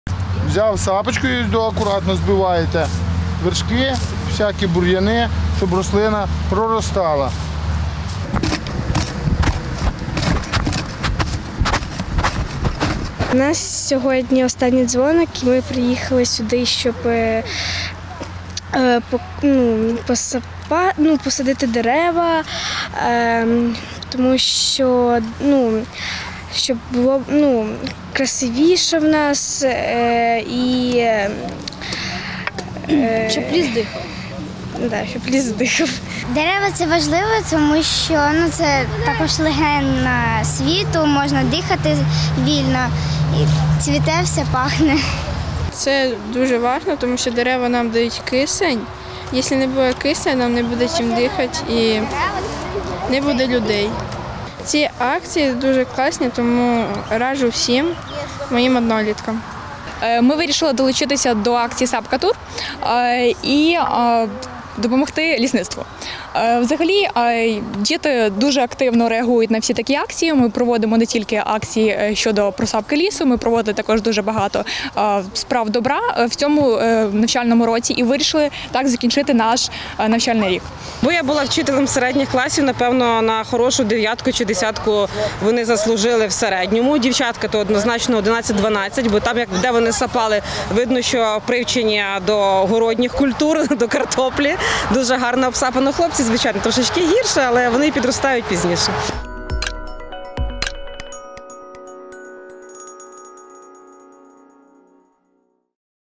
Житомирські учні долучилися до акції «Сапка-тур»_Канал UA: ЖИТОМИР 31.05.19
Учні 5-го класу Житомирської школи №23 замість шкільної «лінійки» долучилися до акції «Сапка-тур».

У Станишівському лісництві діти доглядали за молодими деревами. Як це було – дивіться у сюжеті.